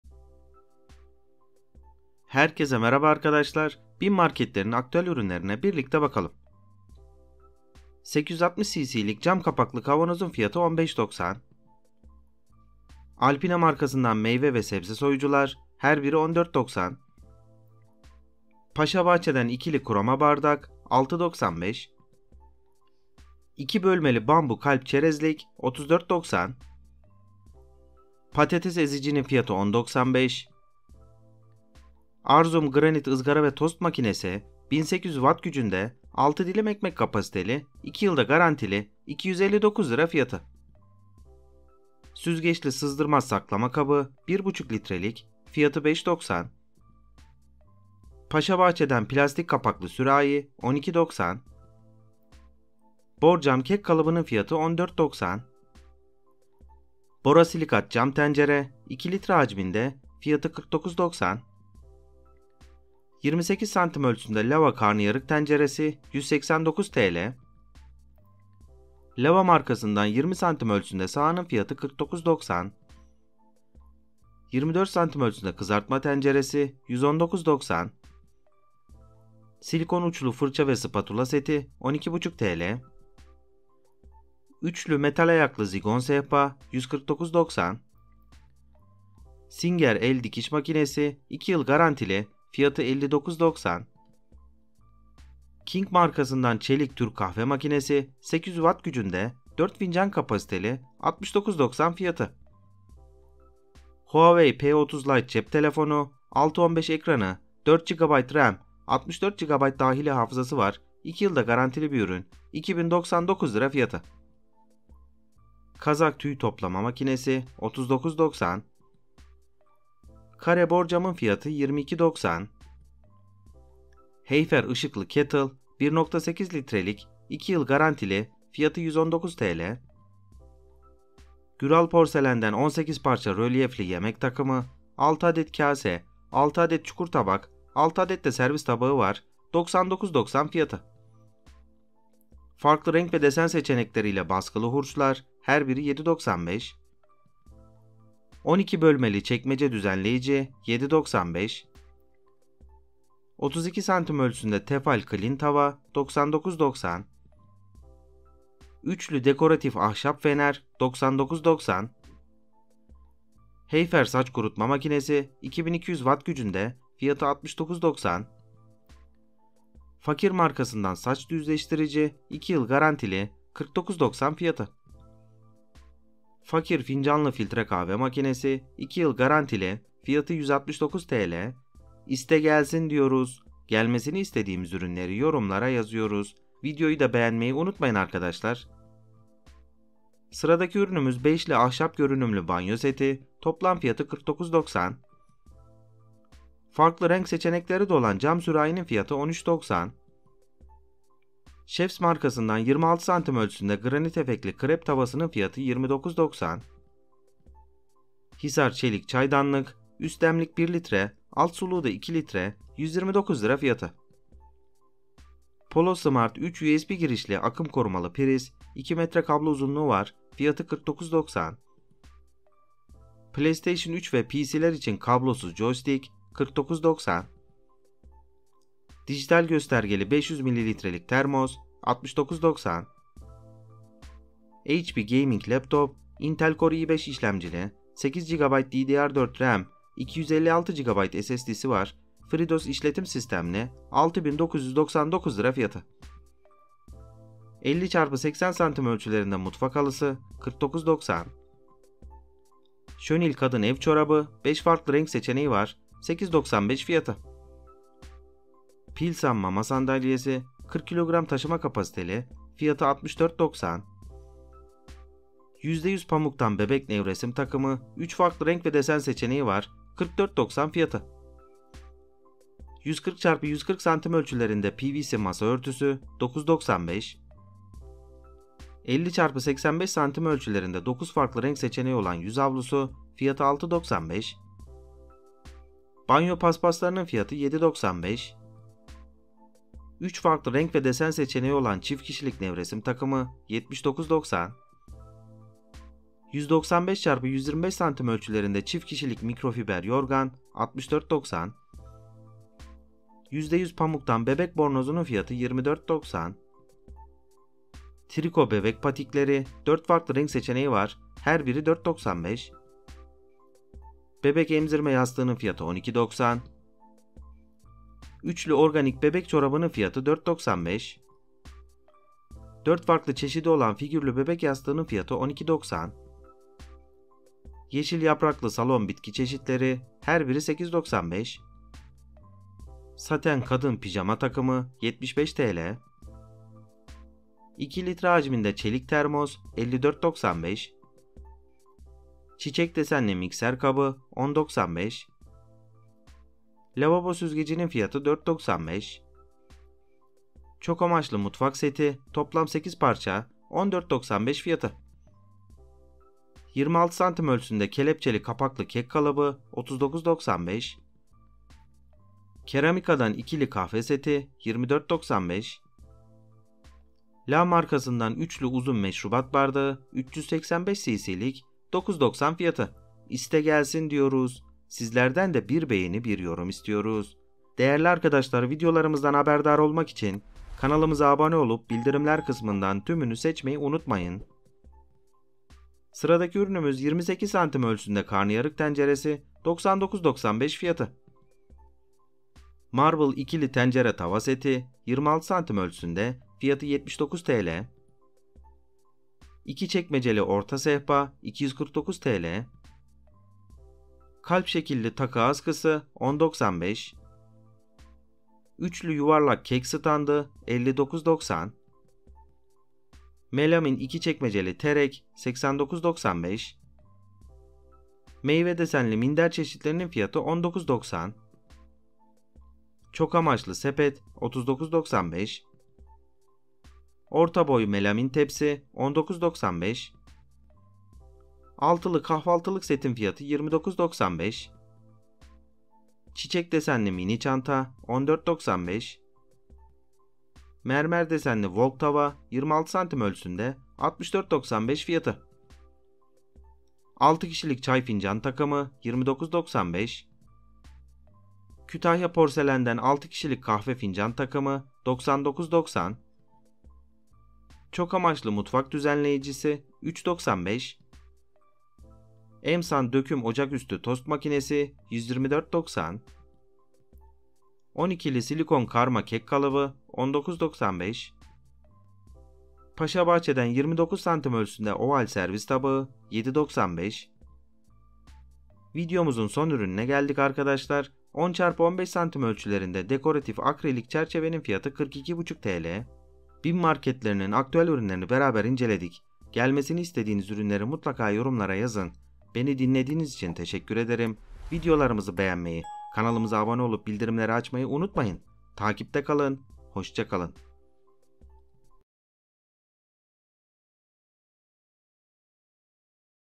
BİM 30 Kasım 2021 Aktüel Ürün Kataloğu | Çeyizlik & Mutfak & Bebek Ürünleri | Beklenen Ürünleri
30 Kasım 2021  BİM marketlerine gelmesini beklediğimiz çeyizlik ve mutfaklar için gerekli aktüel ürünleri sizler için hazırladık. Bim 30 Kasım Haftasında gelebilecek ürünler bim bu hafta  beklenen kataloğunu videomuzdan inceleyebilirsiniz.